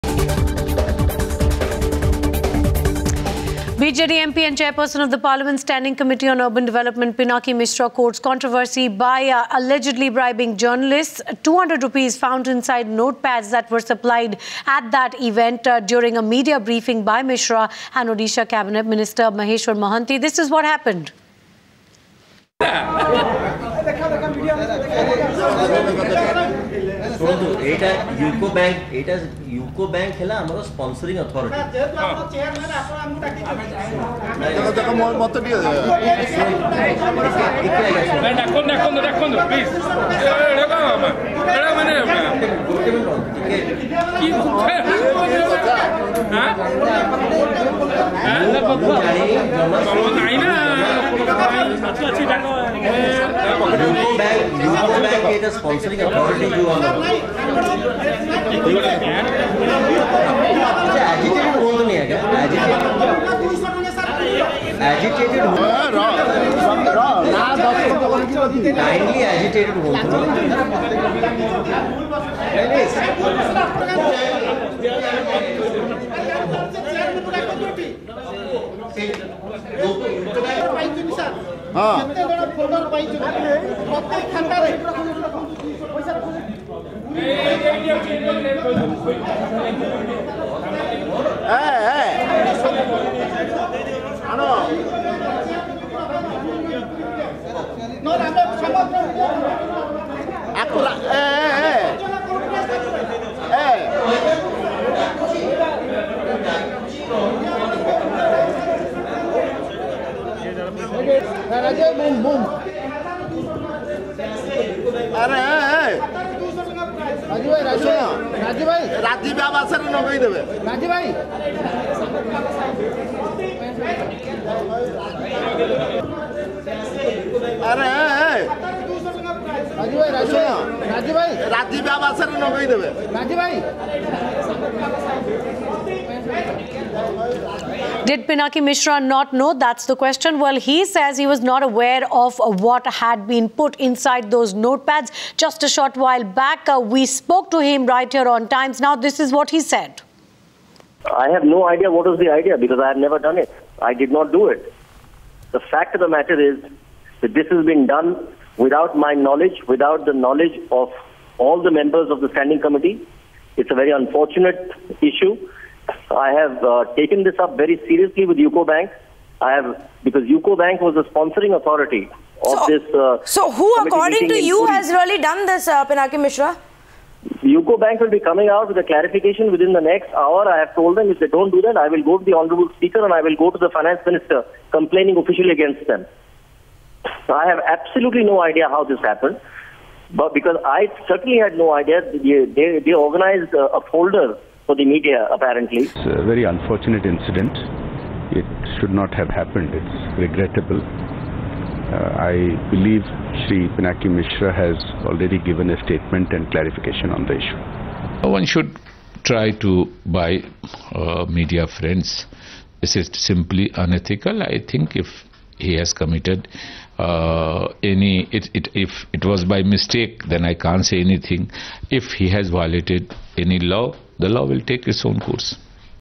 BJD MP & MLA In Notes For News Row - Times Now
Subscription Links :-

Biju Janata Dal (BJD) MP and the Chairperson of Parliament's Standing Committee on Urban Development Pinaki Mishra courts controversy by allegedly bribing journalists. Rs 200 was found inside notepads allegedly supplied by Mishra's supporters during a media briefing by Mishra and an Odisha Cabinet Minister Maheshwar Mahanti. Mishra has admitted the blunder but has pointed fingers at the sponsors. Meanwhile -- Opposition parties have attacked Pinaki for bribing journalists. Social Media Links :-